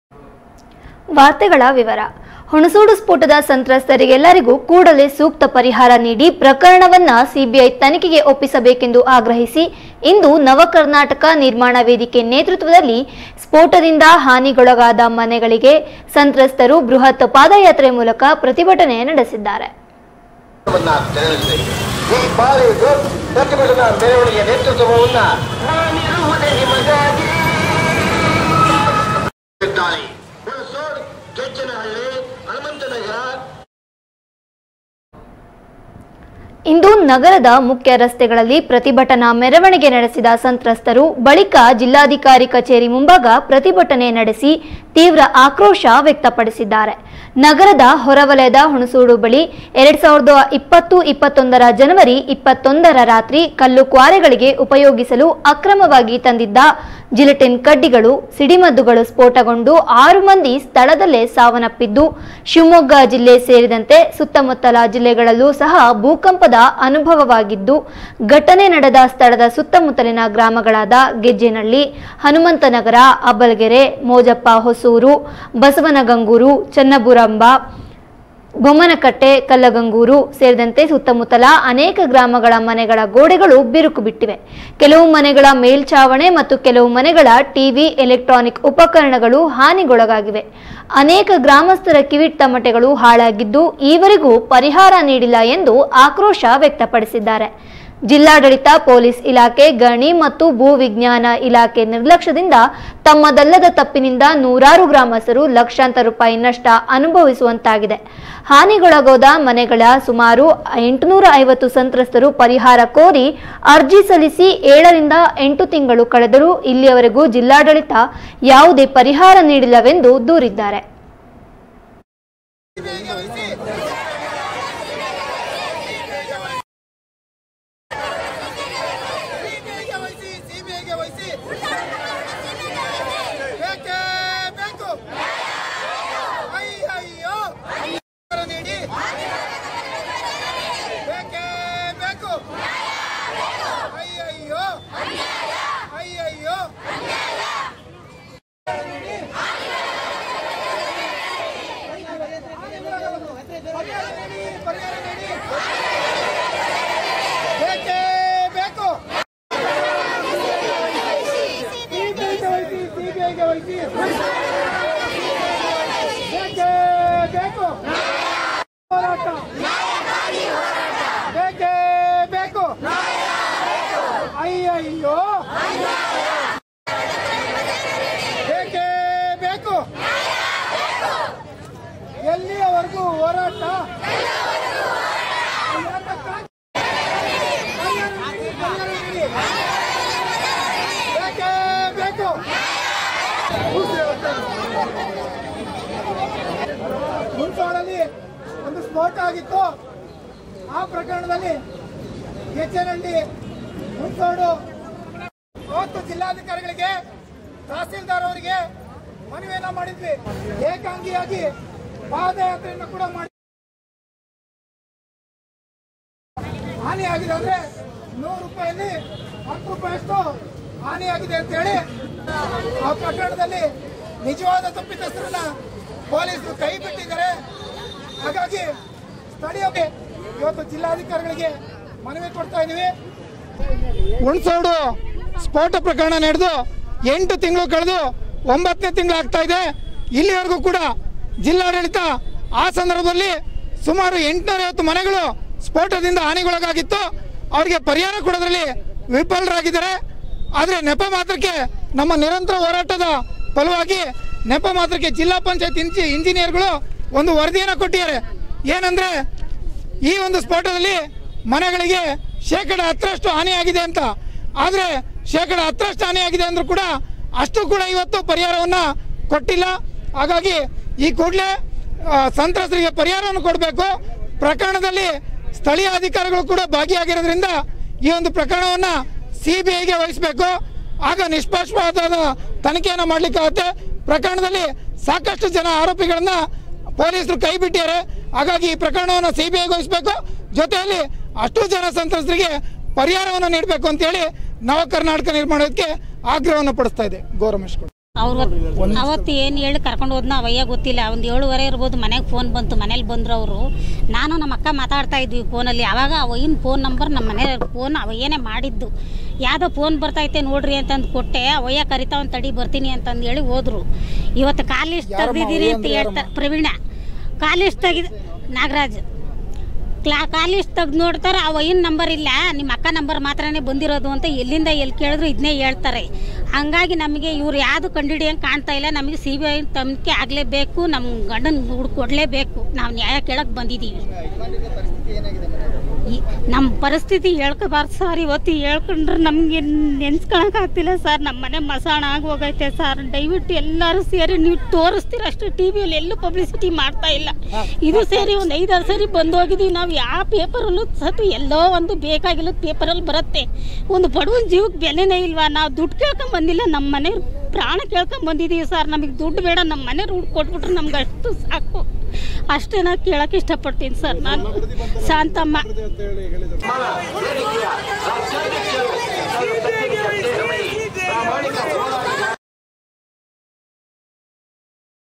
ಹುಣಸೋಡು ಸ್ಫೋಟ ಪ್ರಕರಣ
ಸಂತ್ರಸ್ತರಿಗೆ ಸೂಕ್ತ ಪರಿಹಾರಕ್ಕಾಗಿ ಆಗ್ರಹಿಸಿ ಬೃಹತ್ ಪ್ರತಿಭಟನೆ